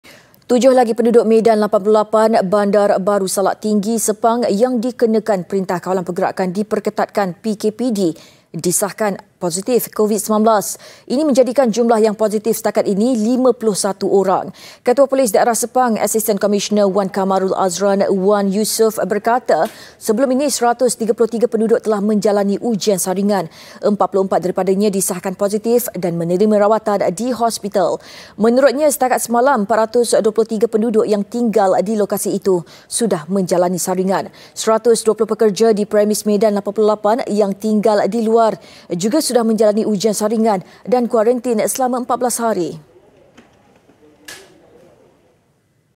COVID-19 | 7 Lagi Penduduk Medan 88 Positif COVID-19
Tujuh lagi penduduk Medan 88, Bandar Baru Salak Tinggi, Sepang yang dikenakan Perintah Kawalan Pergerakan Diperketatkan (PKPD) disahkan positif COVID-19.

 Ini menjadikan jumlah yang positif setakat ini 51 orang.